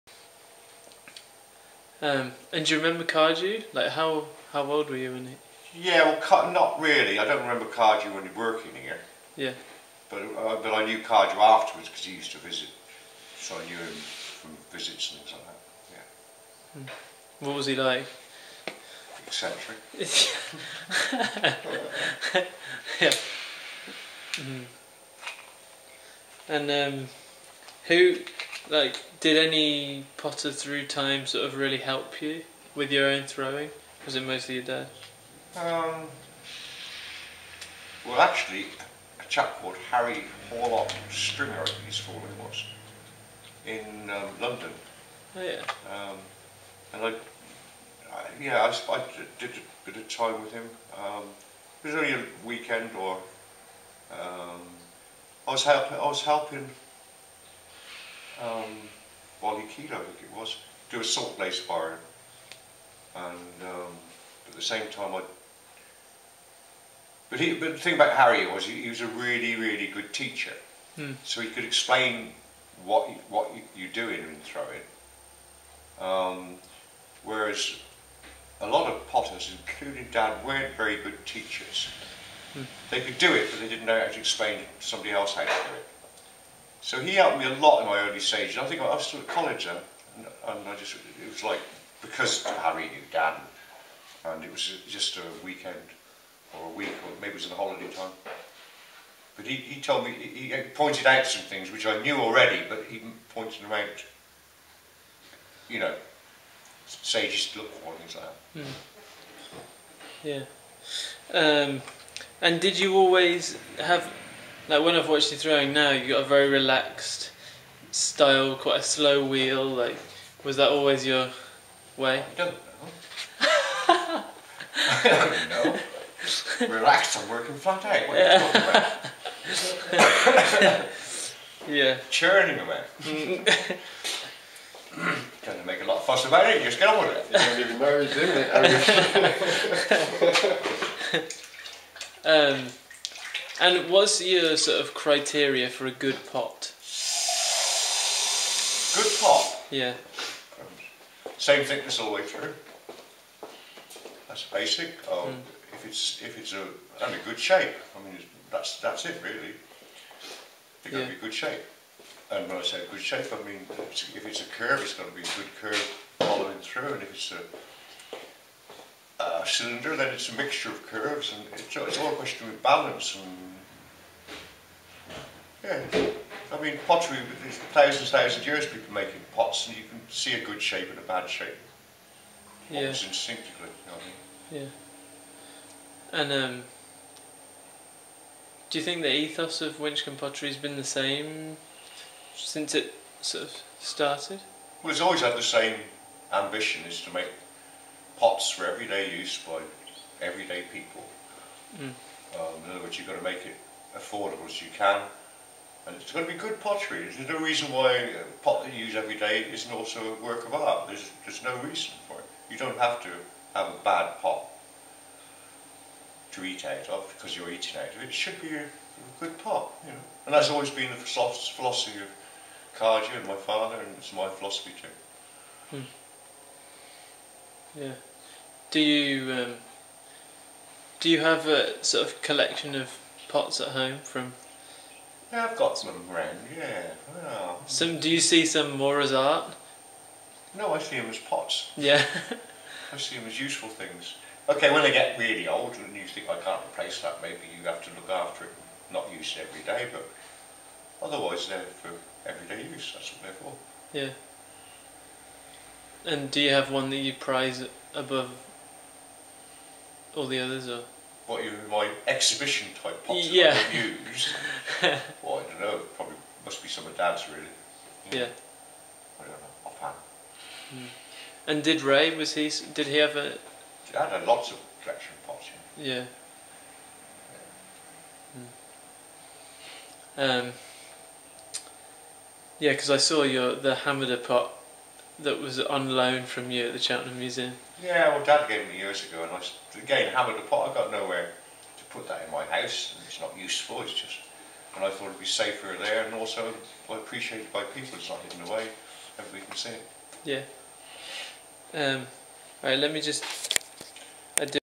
Mike Finch at his wheel. Interview at Winchcombe Pottery, 21/11/13, PART 2
This is the second part of an interview I did with Mike in 2013. I ran out of memory card so its ends a bit abruptly so apologies for that.